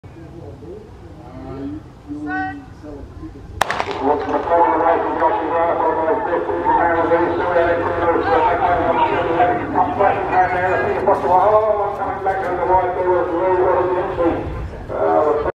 Ht2. 70m Open, Sponsor Kilcoy Global Foods & Kilcoy BP Driveway, Kilcoy Racecourse 26 January 2023
1st. Leroy Dempsey 4.75m 7.72sec, 2nd. Reginald Botchway 4.75m,